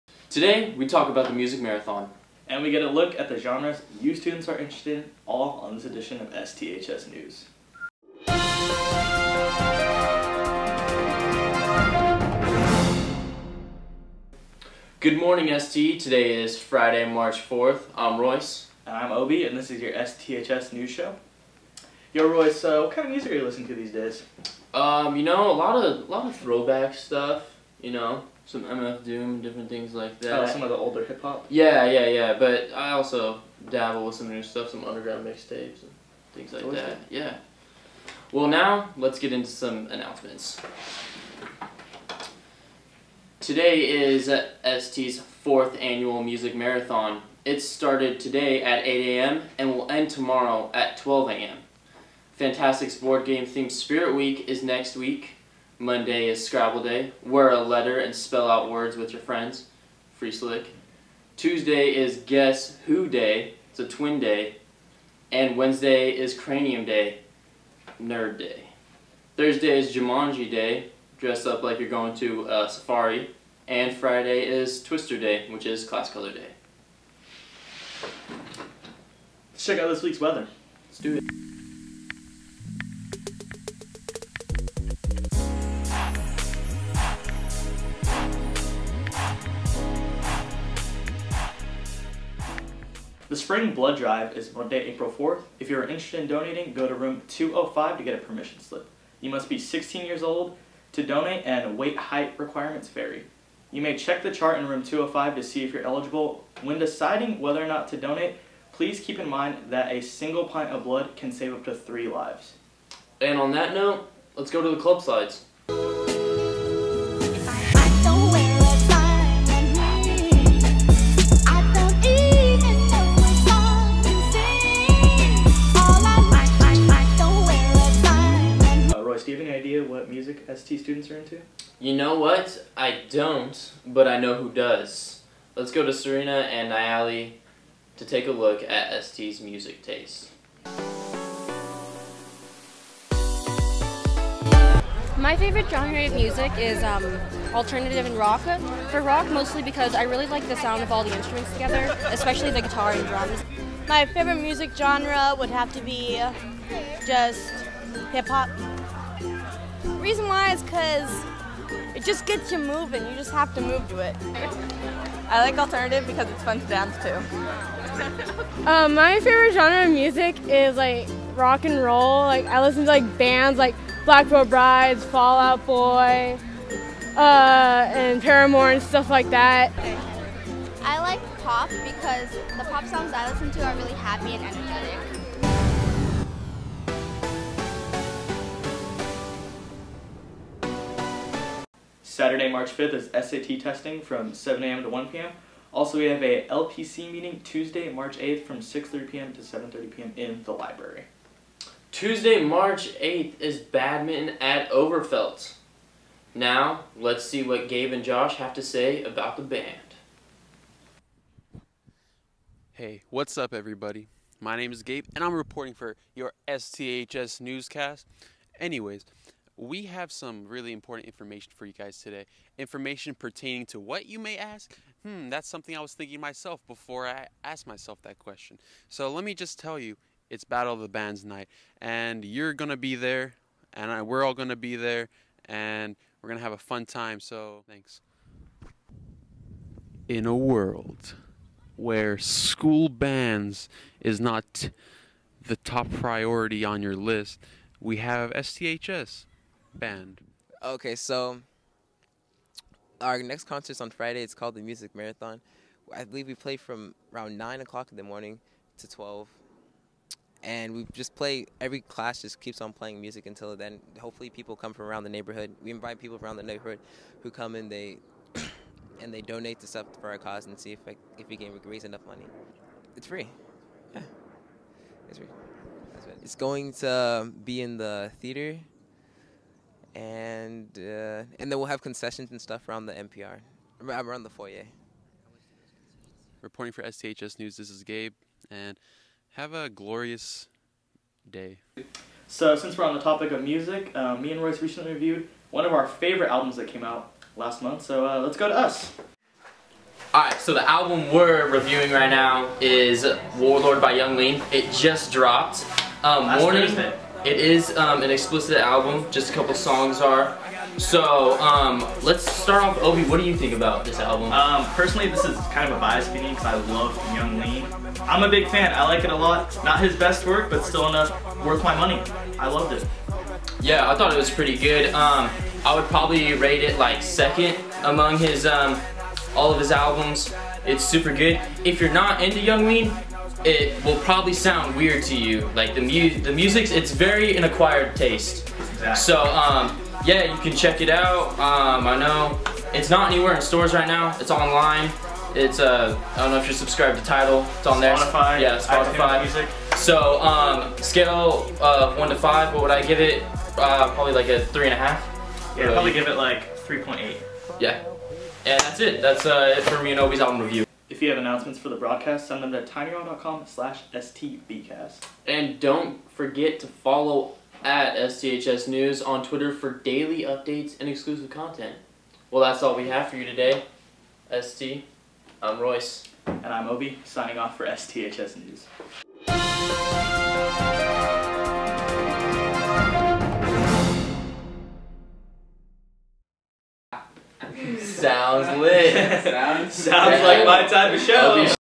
STHS News: March 4th, 2016
Royce and Obie celebrate all things musical in honor of today's Music Marathon!